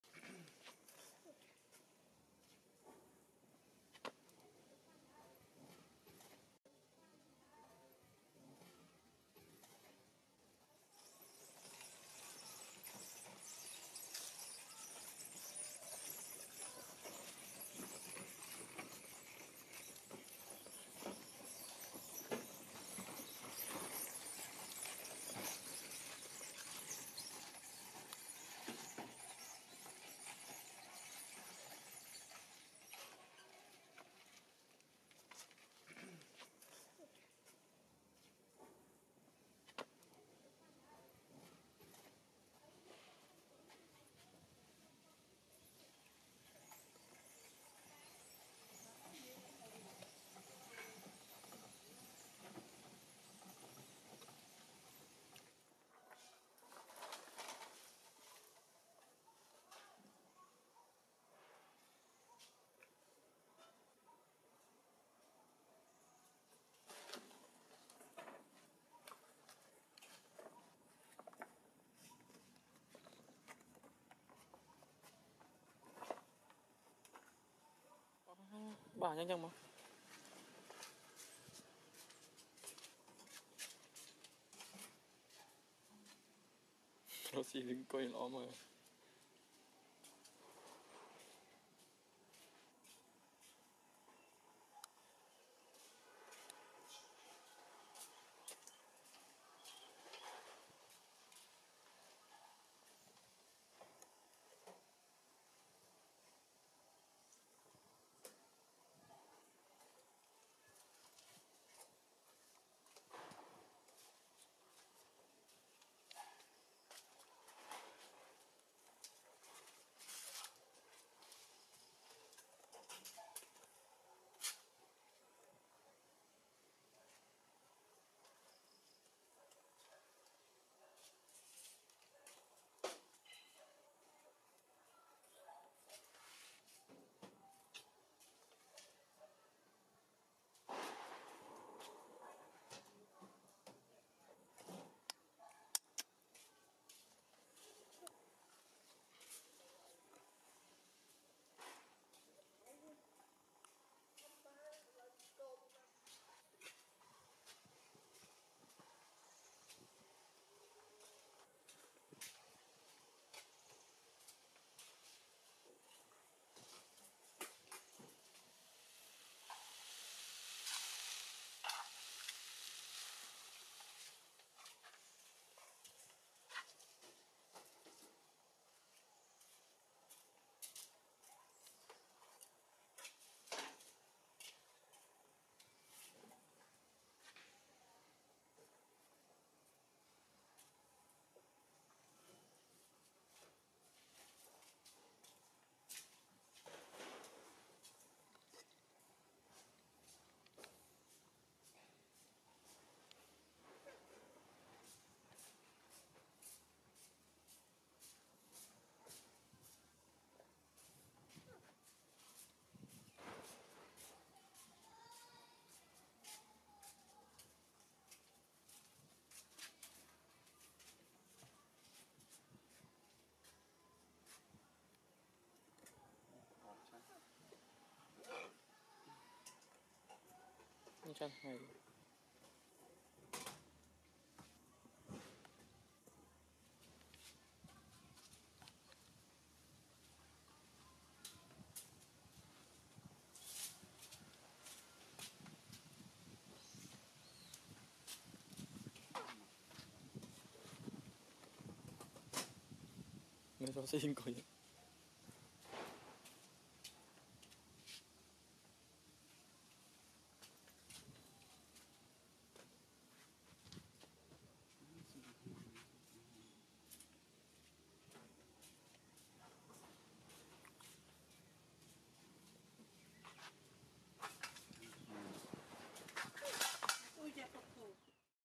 Baby monkey MORA climb short wall _ Lovely little boy monkey
Hello Everyone
This video i want to show you all about:Baby monkey MORA climb short wall _ Lovely little boy monkey
All the video are copyright of My channel ( News Animal ).